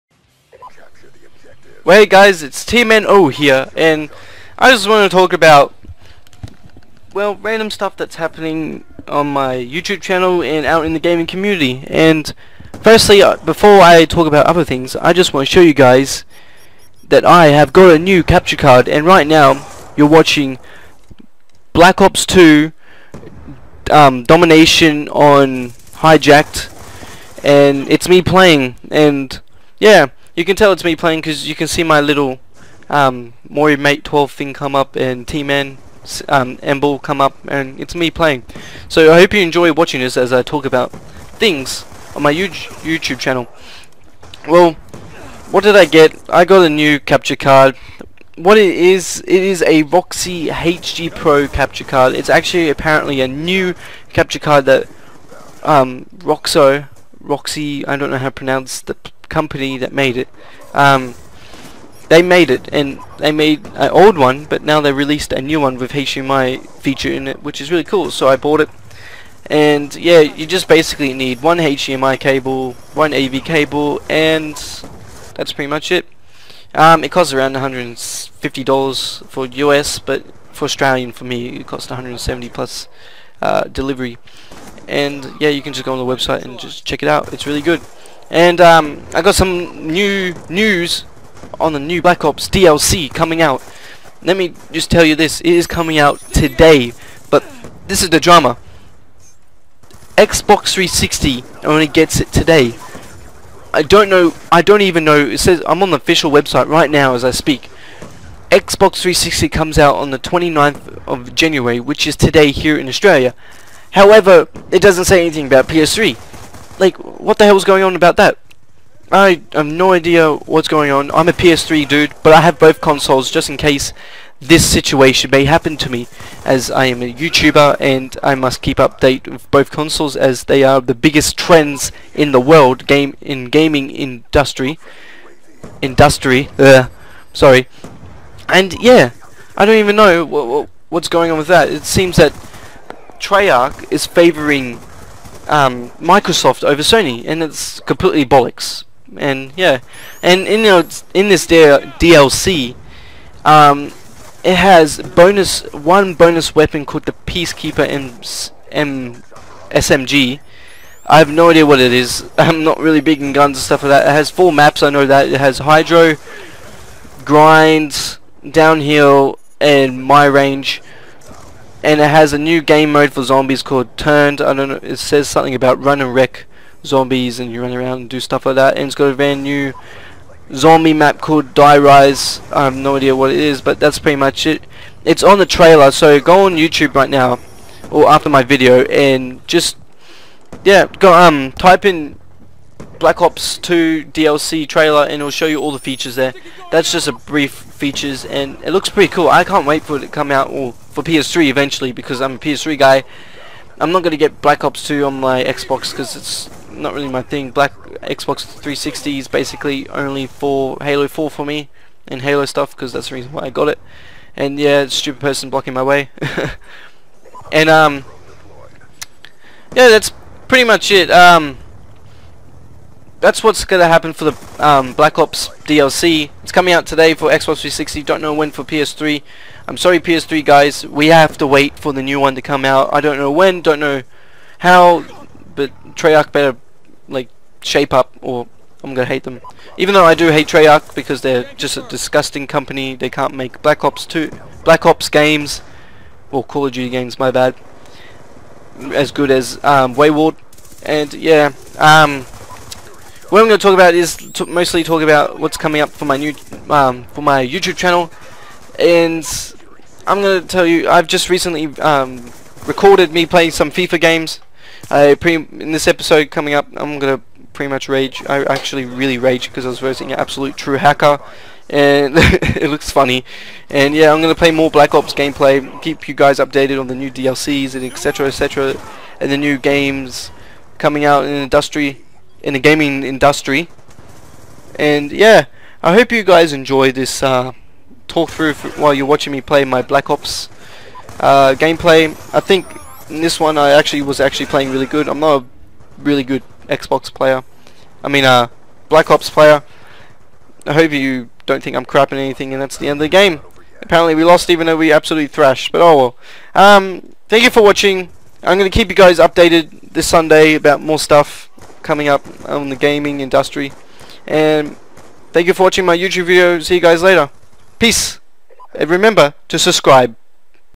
BLACK OPS 2 DLC OUT NOW!
This is information about the new DLC coming out for Black Ops 2. Not the actually gameplay footage.